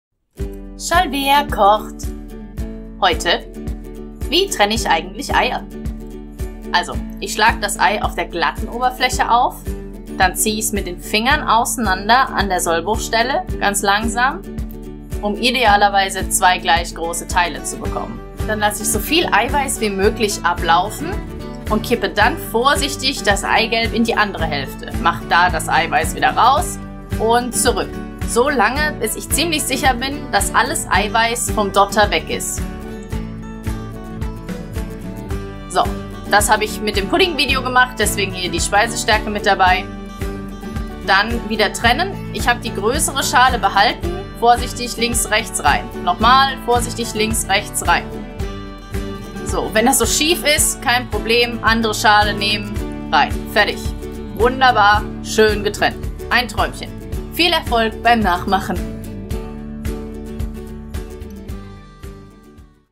Shalvea Kocht - How Too - Eier Trennen
Wenn man nur Eiweis oder Eigelb für ein Rezept braucht und nicht grade in einer Großküche arbeitet wo das separat eingekauft wird, dann muss man die Eier wohl selbst trennen.
Das braucht ein bisschen Fingerspitzengefühl und etwas Übung aber man kann ja mal aus spaß beim Rührei oder Omelett machen die Eier vorher trennen. so als Übung.

Es gibt dafür auf Werkzeuge, aber die führe ich hier nicht vor ;)
da ich dieses Tutorial aus aufnahmen eines anderen Videos geschnitten habe, sind da noch zusätzliche sachen drin. einfach ignorieren. oder anschauen wie man Vanillepudding macht